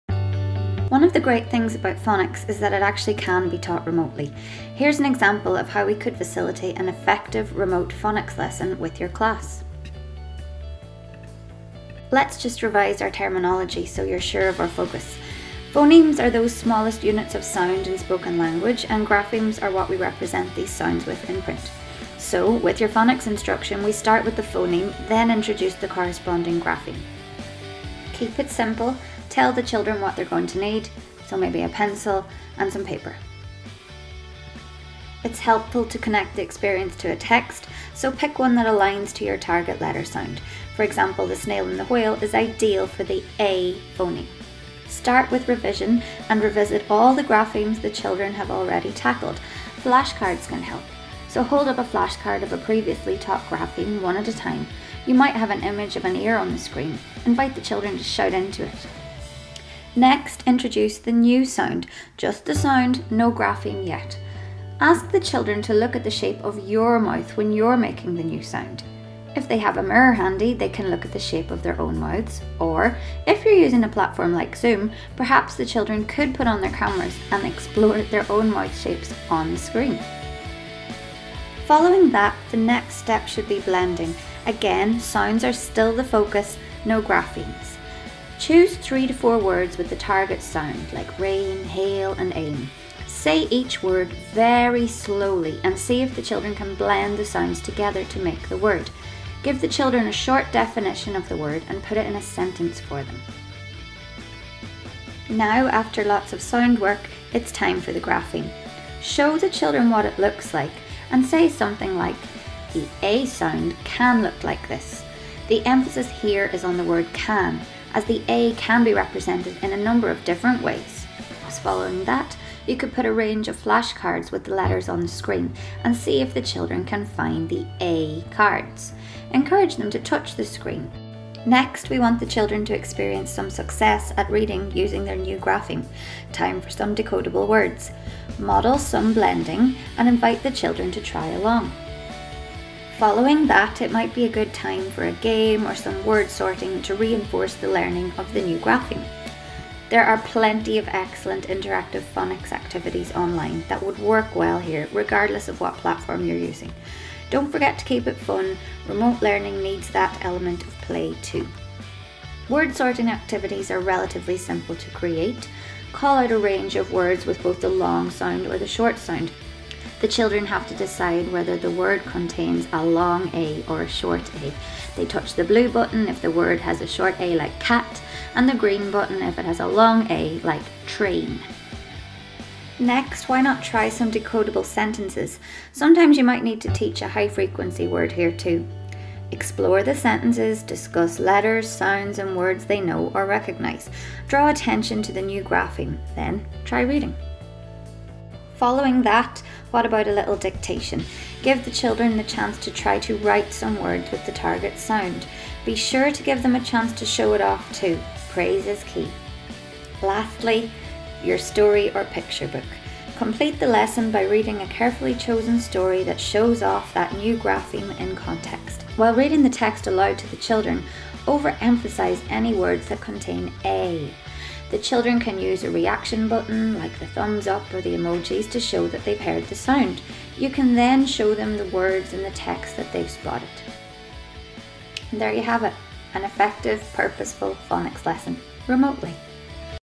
Teaching Phonics Remotely
Explore our suggested approach for remote phonics instruction!

Teaching phonics can still be effective, interactive and purposeful when teaching remotely. Watch our video to learn how.